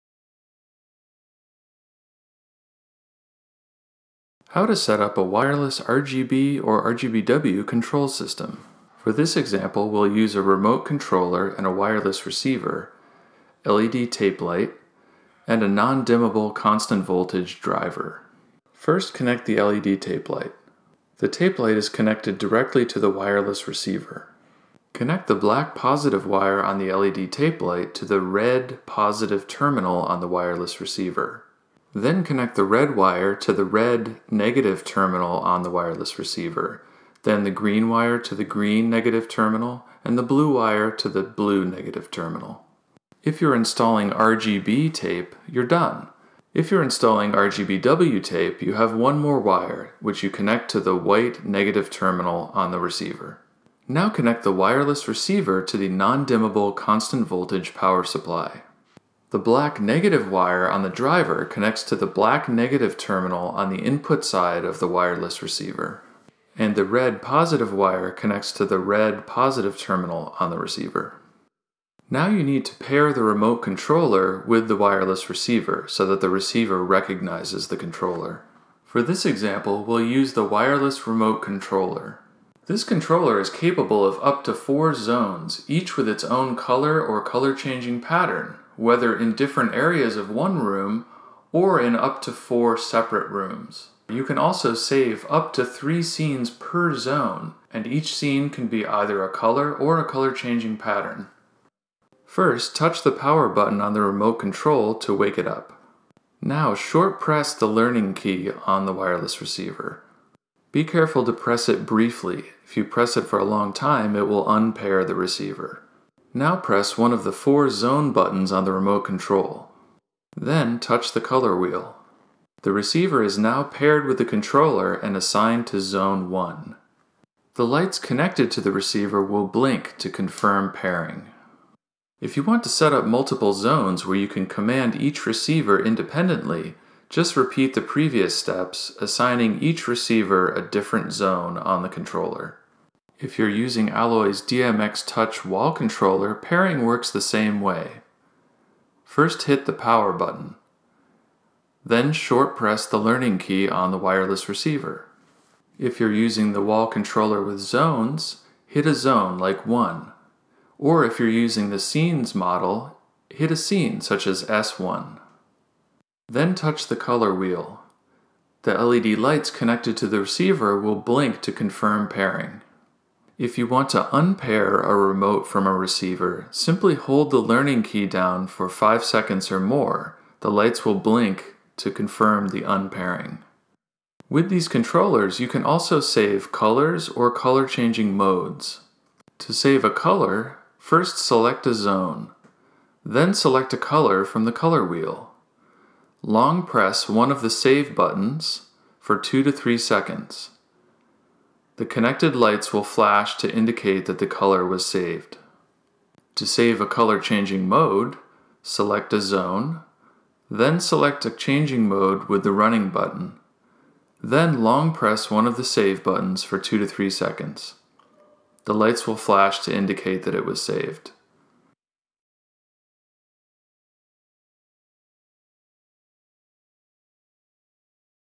How to Set Up a Wireless LED RGB Control System
00:20 - Connect LED tape light to the receiver
00:53 - Connect receiver to power supply
01:12 - Pair the controller to the receiver
02:23 - Pair DMX Touch Wall Controller to the receiver
02:54 - Un-pair a controller from receiver
03:10 - Save a color or color-changing mode